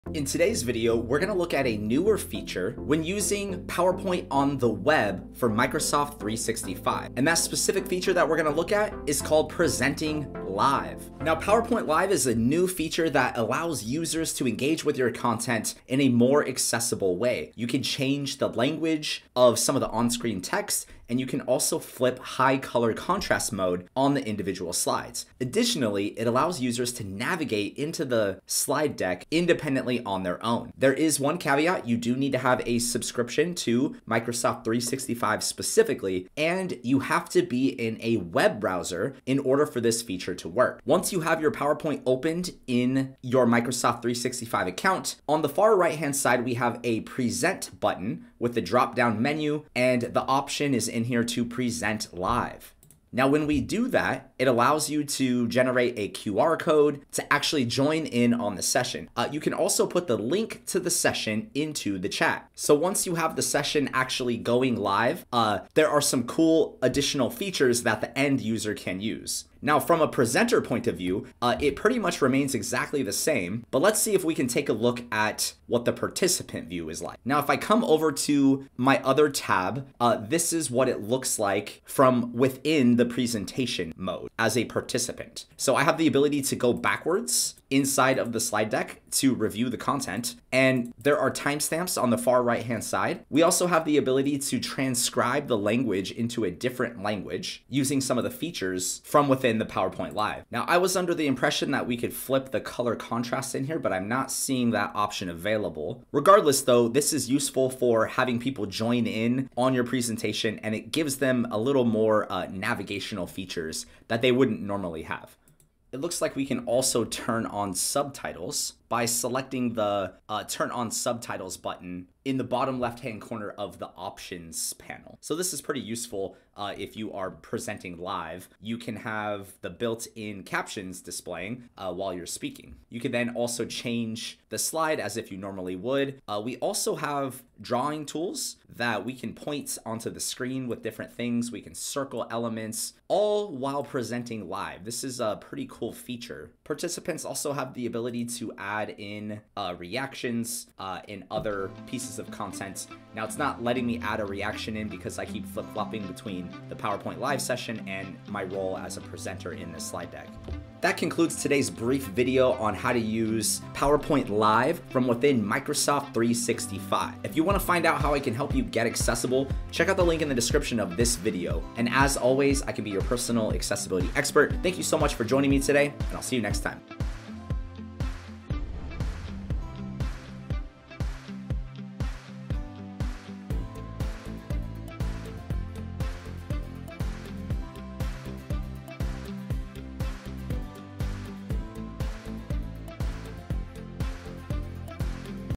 How to use PowerPoint Live in Microsoft 365 Online
This video will walk users through how to use Microsoft PowerPoint Live. It explores features from the point of view of both the presenter and the participant.